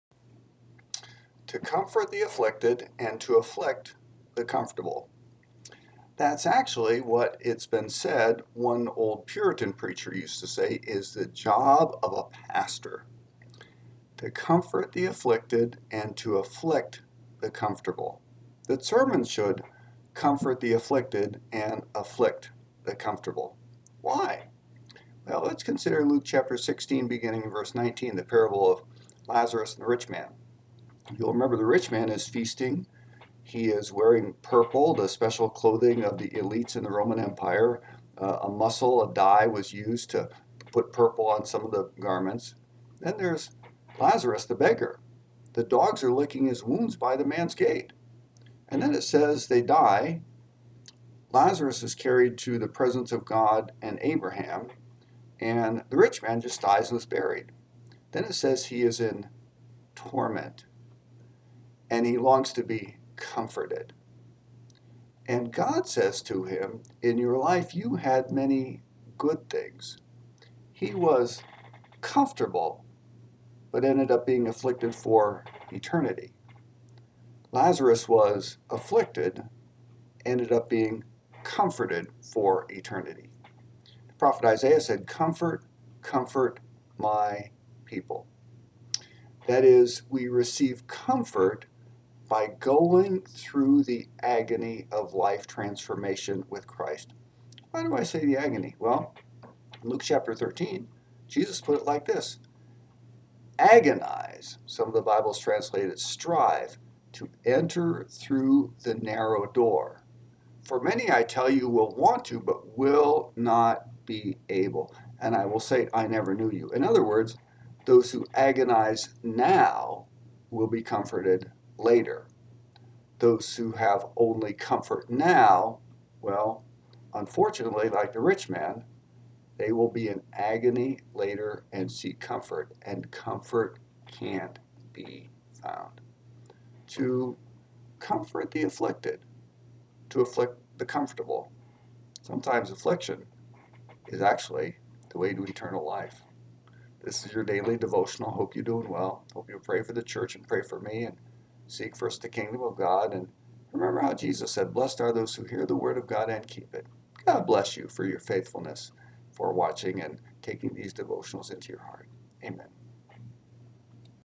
Daily Devotional for July 19, 2024, for North Modesto Church of God and friends.
A daily devotional from Pastor Tim: to afflict the comfortable and comfort the afflicted (Luke 16:19-31).